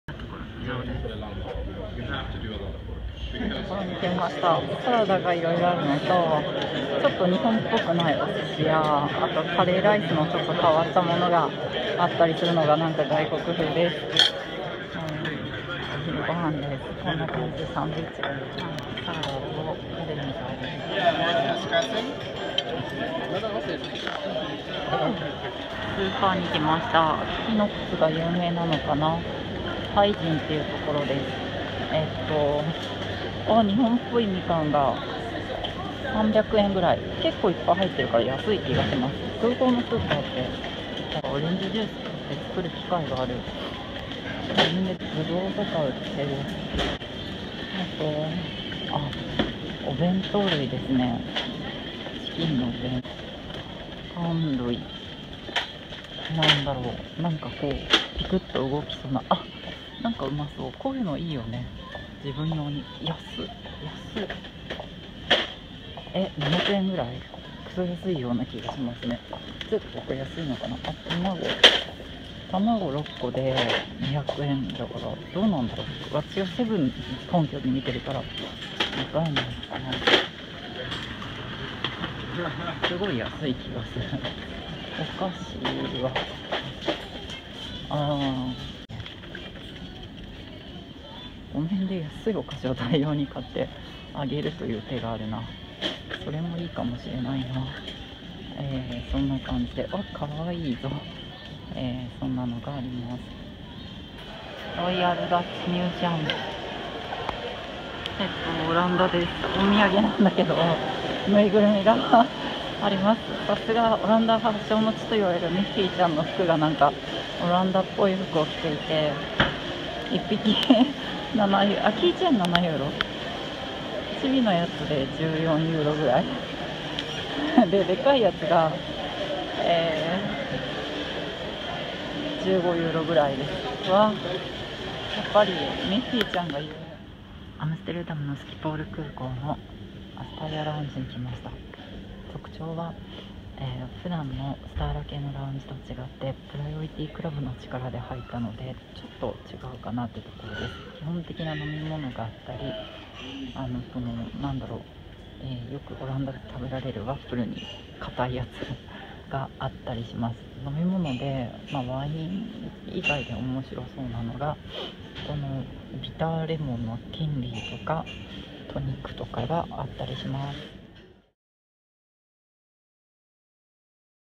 【Yumeneko's stomach】#23 Europe food 2 【Frankfult and Amsterdam】アーネムの食事とフランクフルトとアムステルダム スキポールごはん
夢猫世界を喰らう 23回 アーネムの食事とフランクフルトとアムステルダム スキポールごはん
アーネムで仕事のときのごはん。スキポール空港のスーパーでチーズが安い！のとスキポール空港のお土産とかラウンジとか